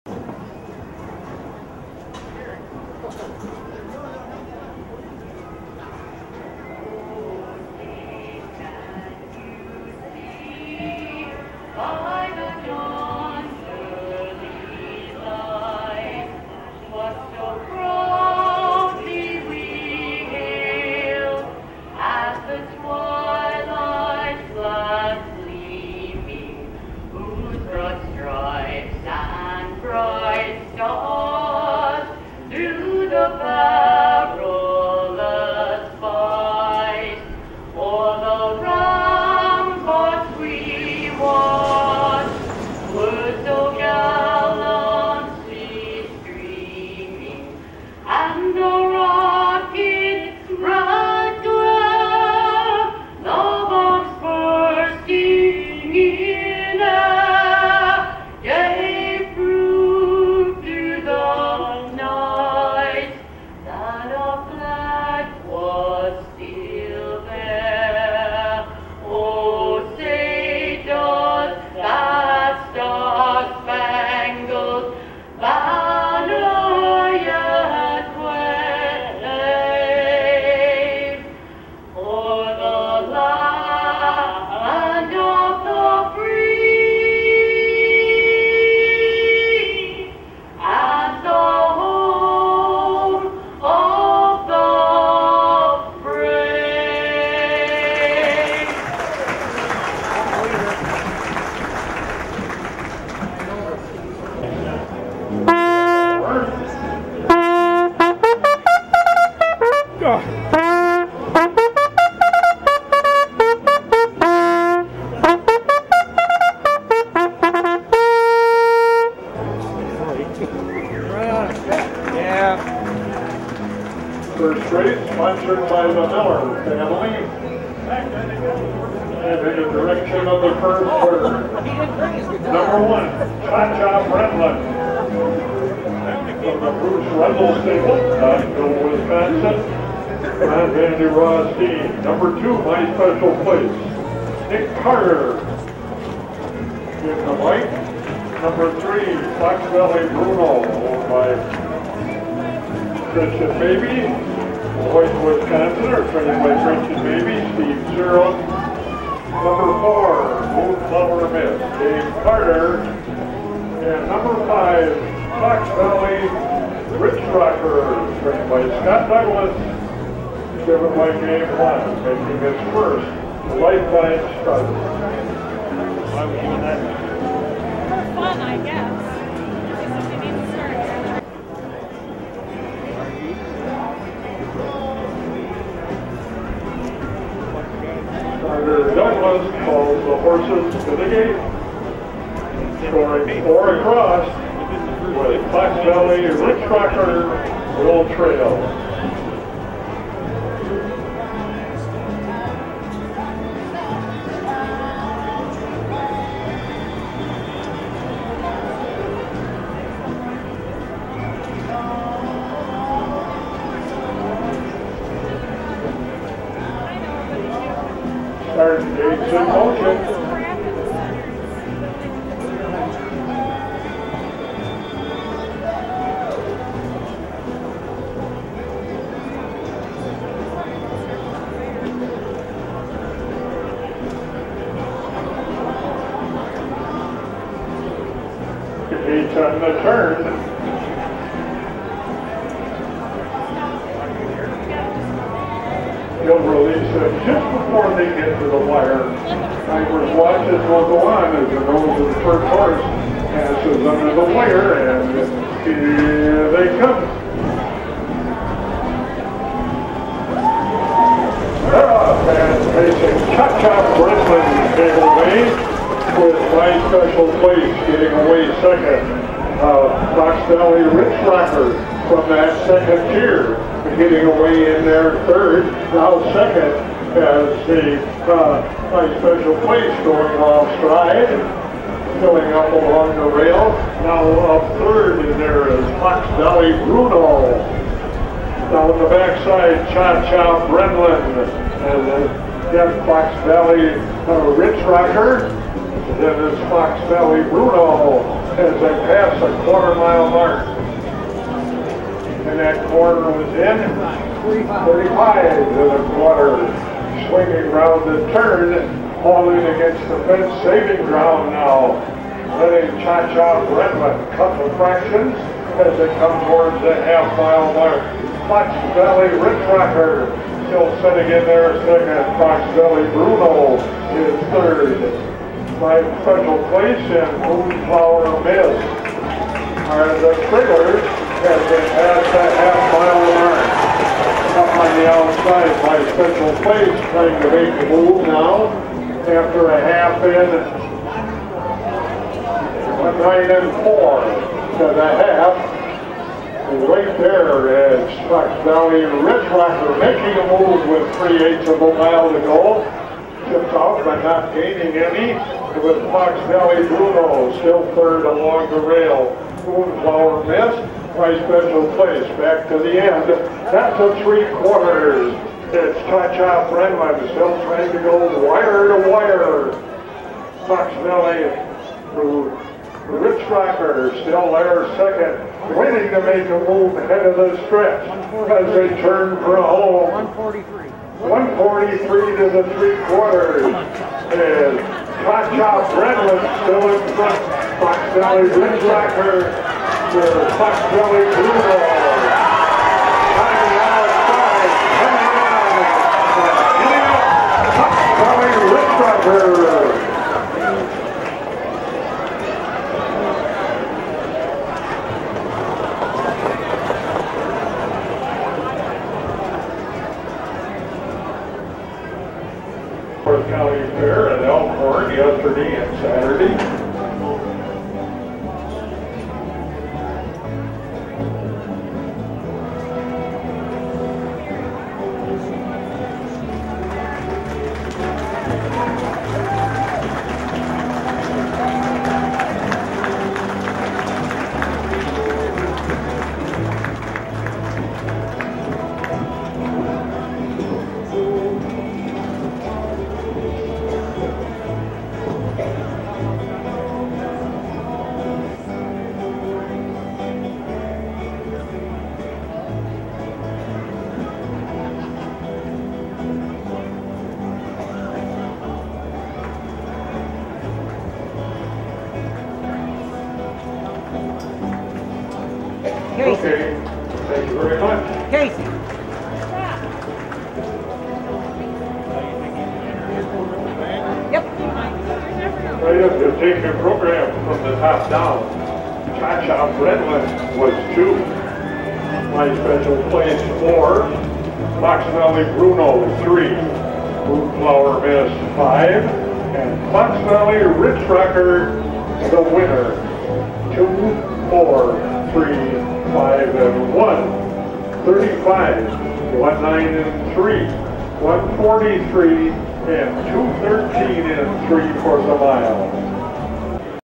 Mineral Point Race#1 09012014
1 MILE PACE RACE 1 PURSE $600
Non-Winners of $375
TIME - 35.0 1:09.4 1:43.0 2:13.3 Track - GD Temp - 82° OFF TIME: 1:00 pm
5 FOX VALLEYRICHROKR 5 1 2:133 NB GWand
1 CHA CHA BRENLIN 1 2 NRBVan De Rostyne
3 FOX VALLEY BRUNO 3 3 NB SSearle
2 MY SPECIAL PLACE 2 4 NB NCarter
4 MOONFLOWER MISS 4 5 NB DCarter